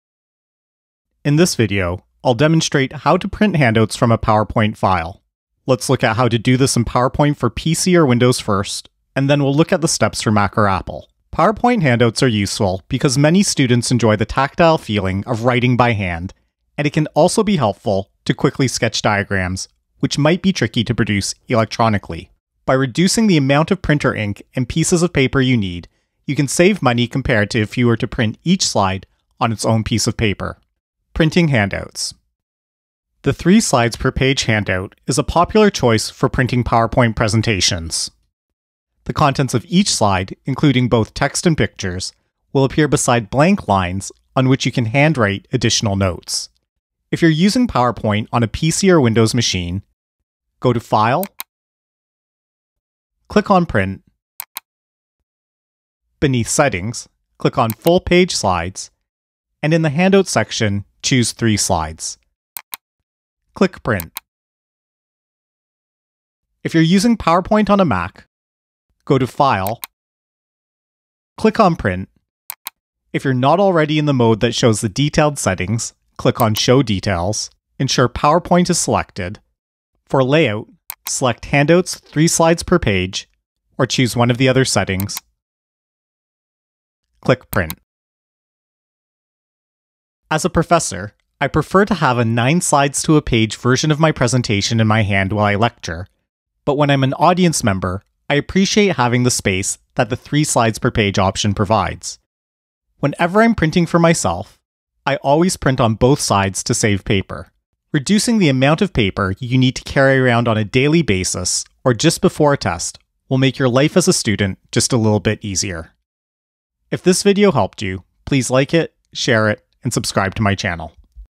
Print Handouts From PowerPoint (PC/Windows + Apple/Mac) -- Learn how in only 2 minutes
College and university students should rarely print all of their lecture slides on individual pages. In most cases, printing three slides to each page with blank lines for hand-written notes is a better option. Don't forget to print on both sides of every page to save paper and ink, which actually permits you to print 6 slides per piece of paper.

Chapters
00:00 - Introduction
00:30 - Printing Handouts
00:46 - Instructions for PC/Windows
01:05 - Instructions for Apple/Mac
01:28 - Closing